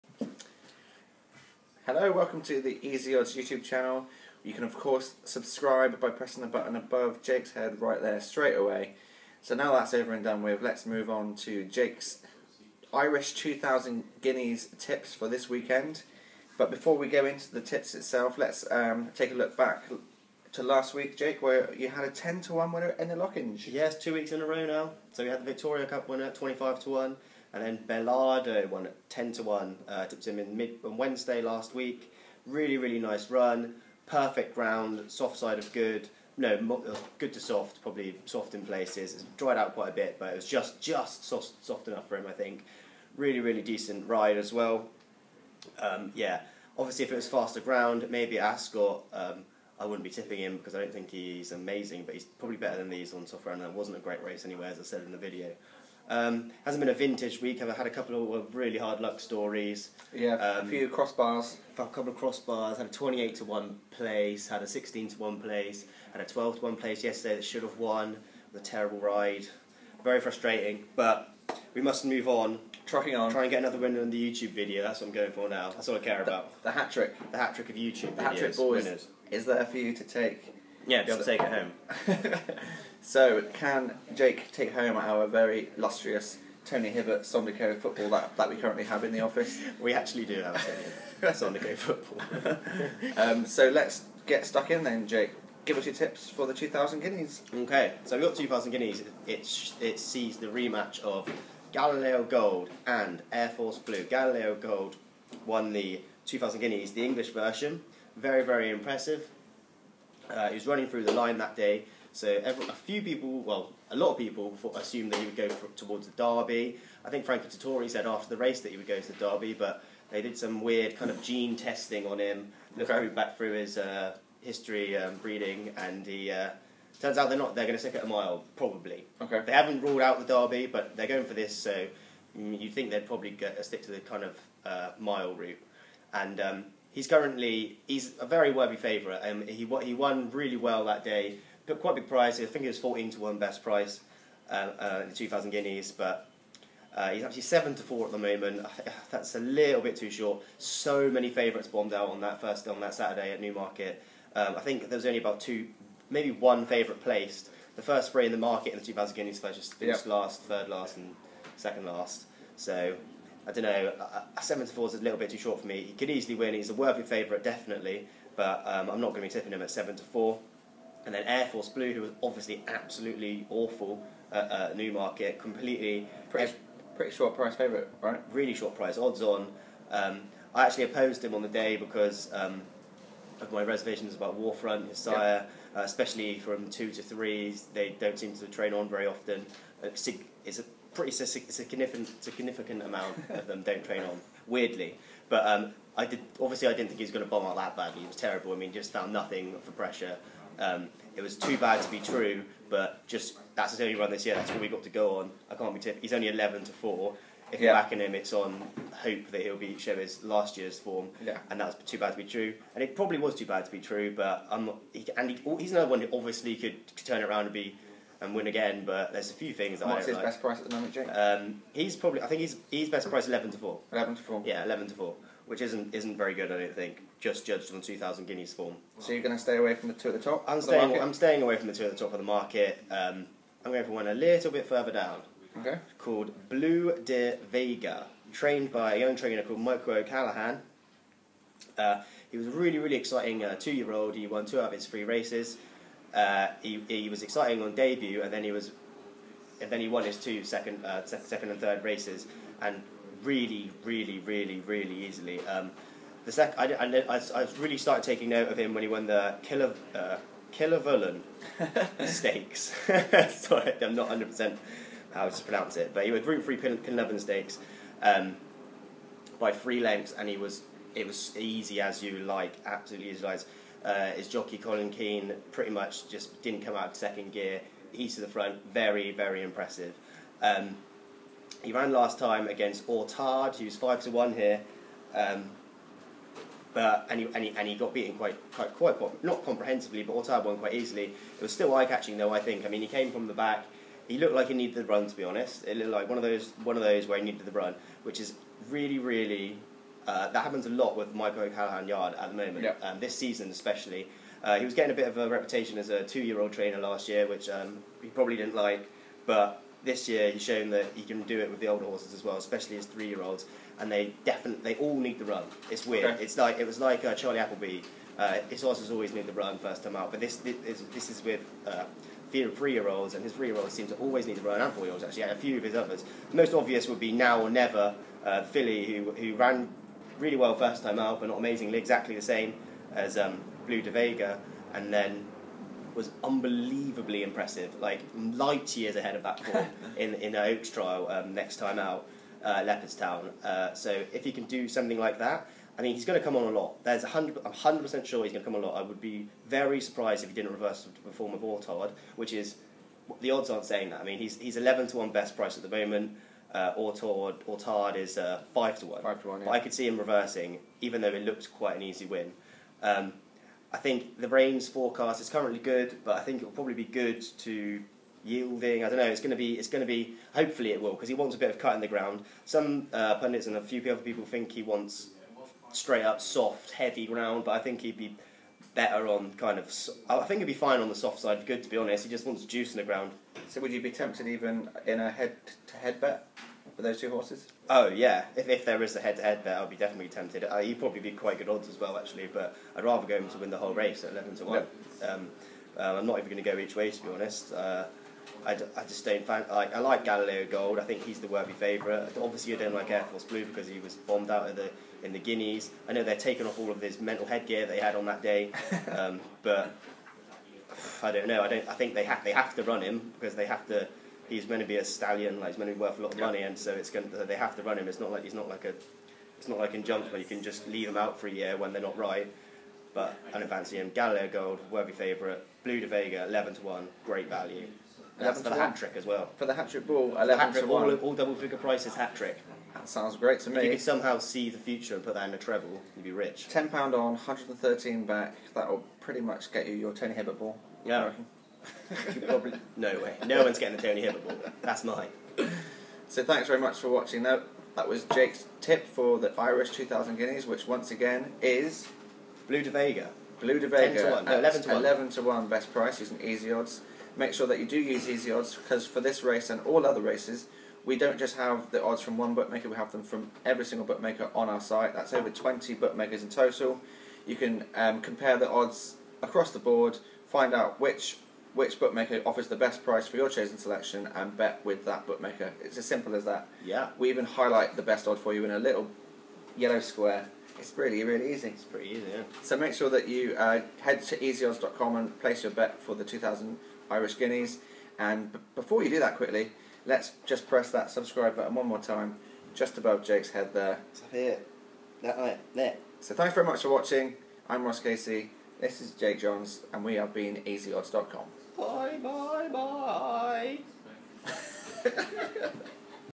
Jake's Horse Racing Tips - Irish 2000 Guineas Tips
Irish 2000 Guineas Tips

Follow Jake on Twitter @JakeJohns

Our guy Jake Johns is on for a hat-trick of winners in these vids after finding the Victoria Cup winner at 25/1 and Lockinge winner at 10/1! Here he and Ross Casey discuss the Group One Irish 2000 Guineas at The Curragh on Saturday.